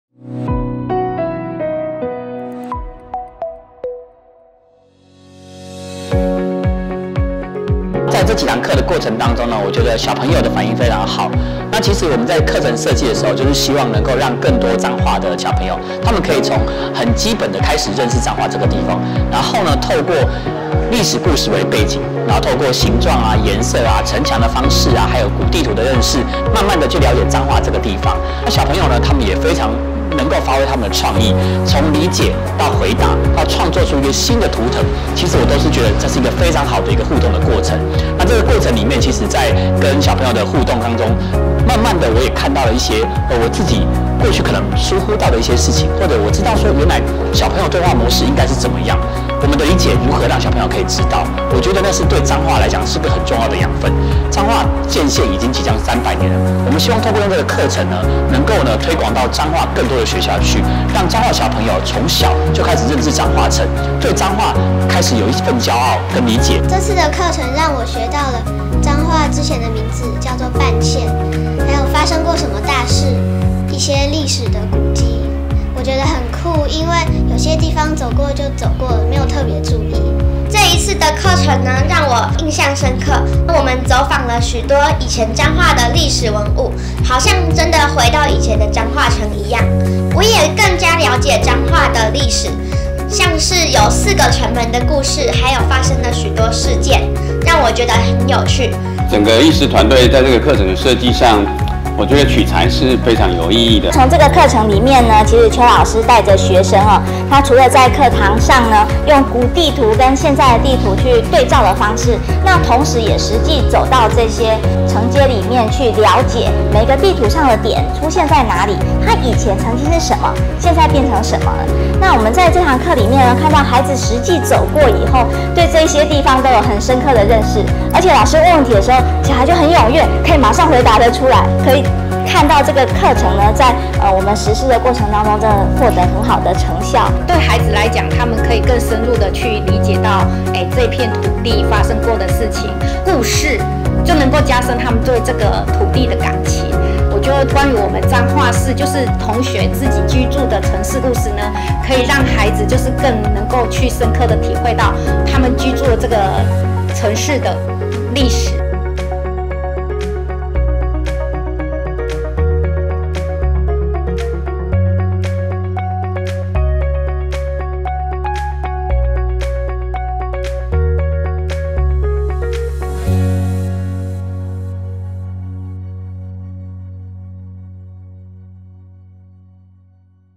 111年度文化體驗教育推廣 【未來地圖 吾線可期彰化古城街廓再現】
藝師團隊/ 未來地圖有限公司 授課學校/

透過一份彰化城的古地圖，穿越建城之初的歷史長河，對照當前迥異的繁華街景圖，從中觀察、比較今昔的異同，之後再透過實地走查，親臨每個地圖上的重要標的。